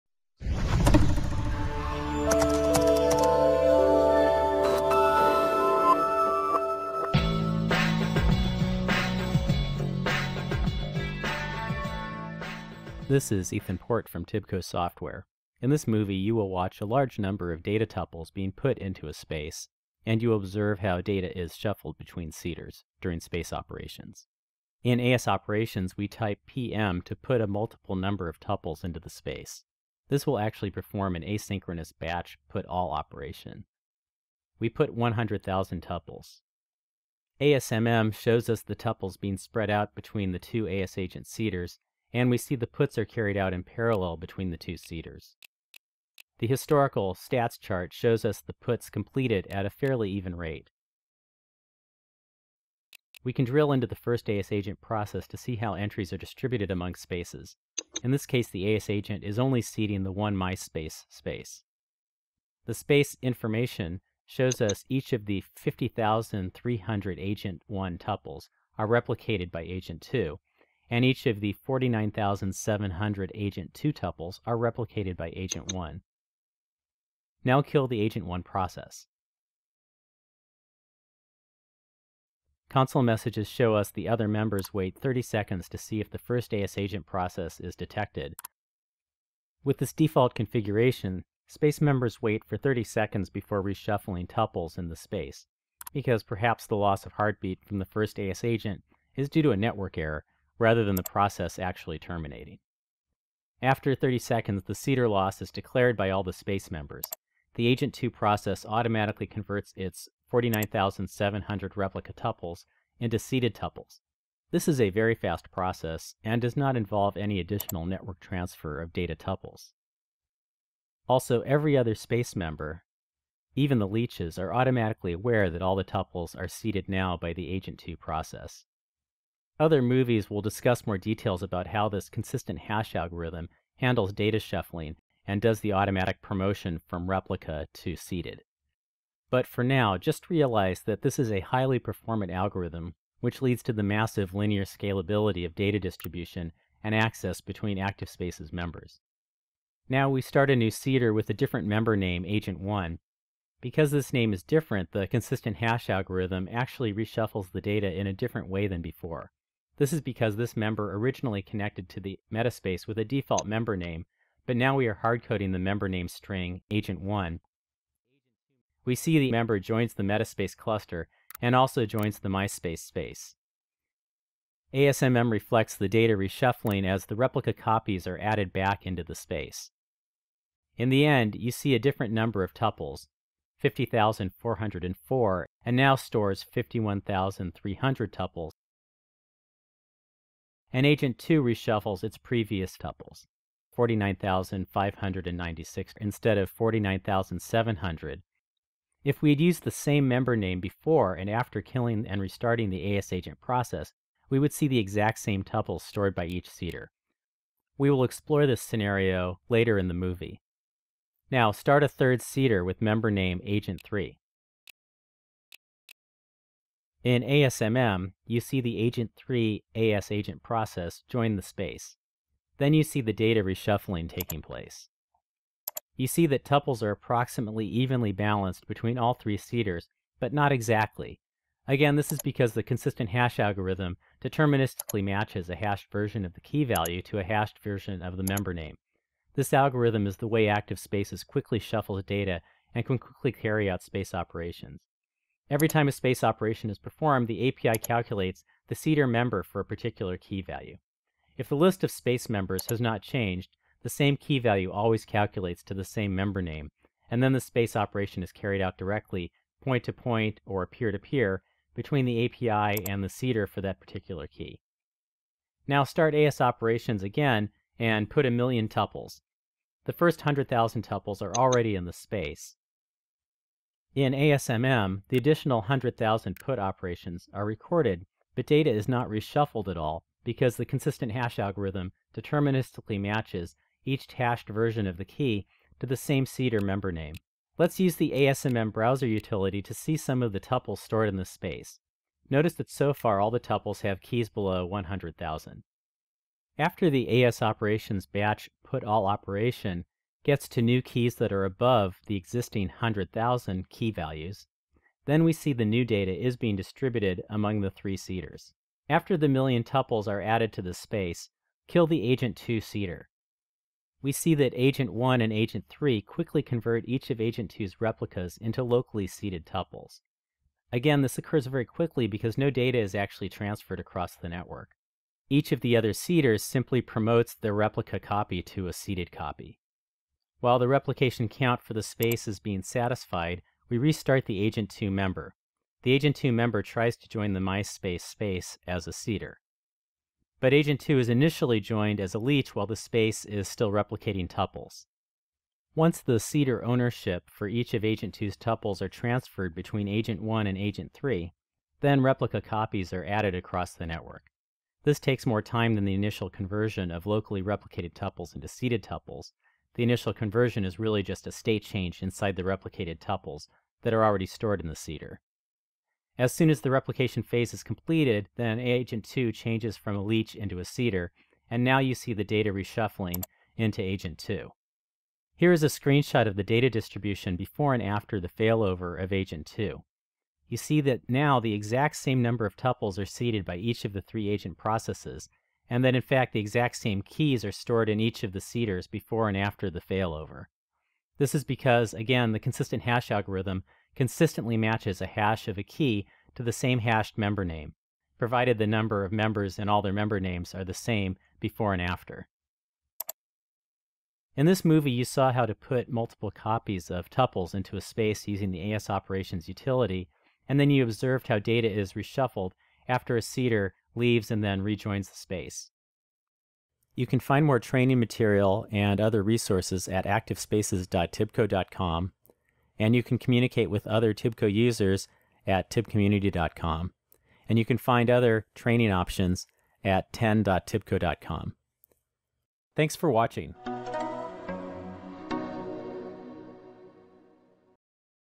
Analyze Batch Operations Behavior - TIBCO ActiveSpaces v2.1.x
In this movie you learn the steps to observe the effects of batch operations on seeders in TIBCO ActiveSpaces v2.1.x. The ASOperations Java example is used to PUT 100,000 data tuples into a Space. In the ASMM GUI you observe how the tuples are distributed between two as-agent seeder processes, and you observe how data is replicated.

Next you see how data is reshuffled when a seeder leaves a Space, then rejoins with a different metaspace member name.

Then one million Tuples are PUT into the Space, and a third Seeder is added.

Finally you again observe how data is reshuffled when a seeder leaves then rejoins a Space with the same member name.

This official training movie is created by the TIBCO Education Network (TEN).

Pathway: JS-TAS1002 Jump Start: Using the Java Examples and Utilities in TIBCO ActiveSpaces v2.x.

TAS7715 - TAS7716 - TAS7717 - TAS7718 - TAS7719